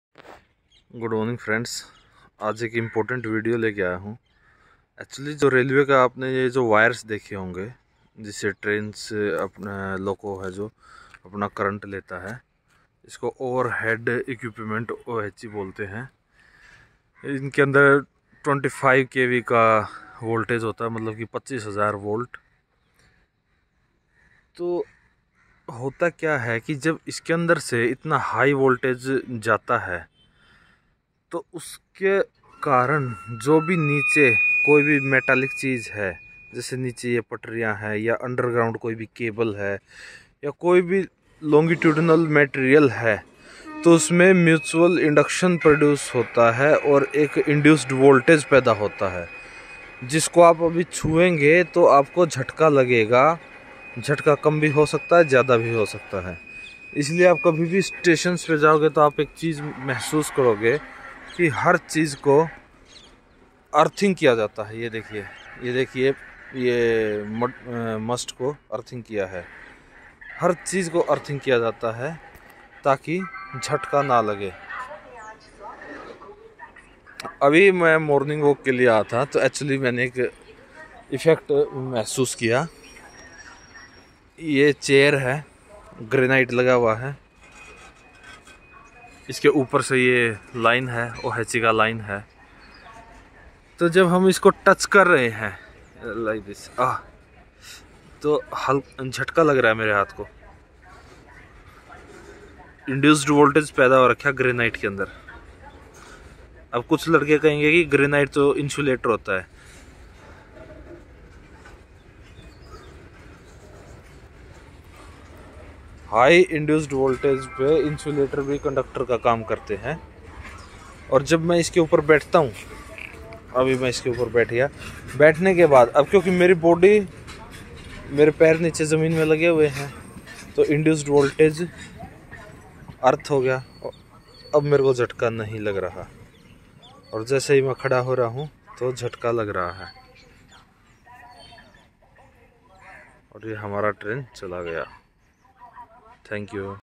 Indian Railway OHE effect . Induced voltage ⚡ effect. रेल की लाइन के पास झटका कैसे लगाता है!
When a conductor comes near to high voltage carring conductor an induced voltage is produced.